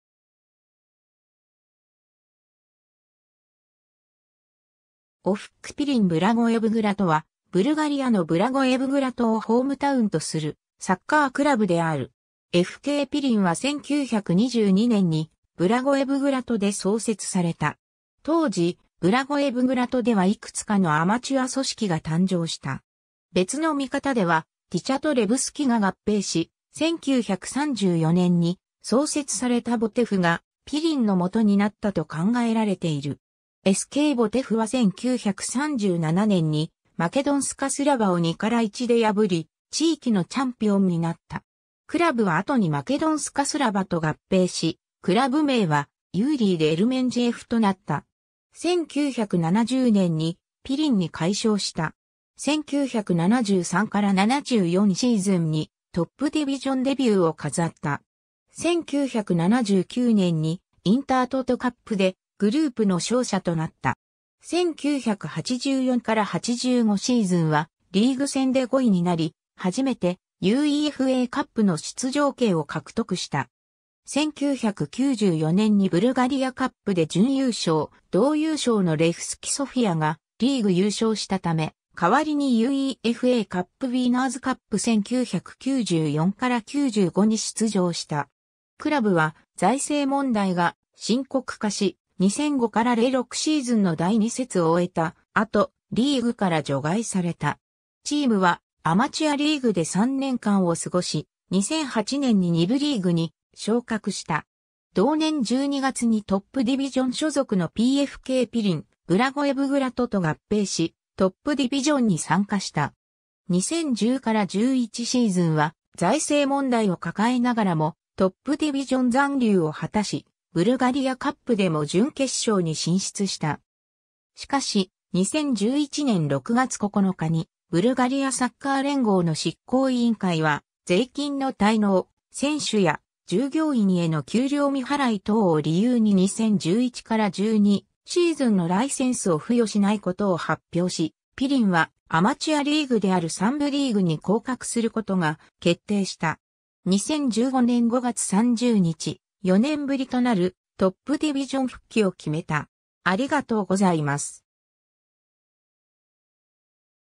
OFKピリン・ブラゴエヴグラト
OFKピリン・ブラゴエヴグラト（ОФК Пирин Благоевград）は、ブルガリアのブラゴエヴグラトをホームタウンとするサッカークラブである。
 FKピリンは1922年にブラゴエヴグラトで創設された。
当時、ブラゴエヴグラトではいくつかのアマチュア組織が誕生した。
別の見方では、ティチャとレヴスキが合併し、1934年に創設されたボテフがピリンの基になったと考えられている。
SKボテフは1937年にマケドンスカ・スラヴァを2-1で破り、地域のチャンピオンになった。
クラブは後にマケドンスカ・スラヴァと合併し、クラブ名はユーリイ・デルメンジエフとなった。
1970年にピリンに改称した。
 1973-74シーズンにトップディビジョンデビューを飾った。
1979年にインタートトカップでグループの勝者となった。
1984-85シーズンはリーグ戦で5位になり、初めてUEFAカップの出場権を獲得した。
1994年にブルガリア・カップで準優勝、同優勝のレフスキ・ソフィアがリーグ優勝したため、代わりにUEFAカップウィナーズカップ 1994-95に出場した。
 クラブは財政問題が深刻化し、2005-06シーズンの第2節を終えた後、リーグから除外された。
チームはアマチュアリーグで3年間を過ごし、2008年に2部リーグに昇格した。
同年12月にトップディビジョン所属のPFKピリン・ブラゴエヴグラトと合併し、トップディビジョンに参加した。
 2010-11シーズンは財政問題を抱えながらも、トップディビジョン残留を果たし、ブルガリア・カップでも準決勝に進出した。
しかし、2011年6月9日にブルガリアサッカー連合の執行委員会は、税金の滞納、選手や従業員への給料未払い等を理由に2011-12シーズンのライセンスを付与しないことを発表し、ピリンはアマチュアリーグである3部リーグに降格することが決定した。
2015年5月30日、4年ぶりとなるトップディビジョン復帰を決めた。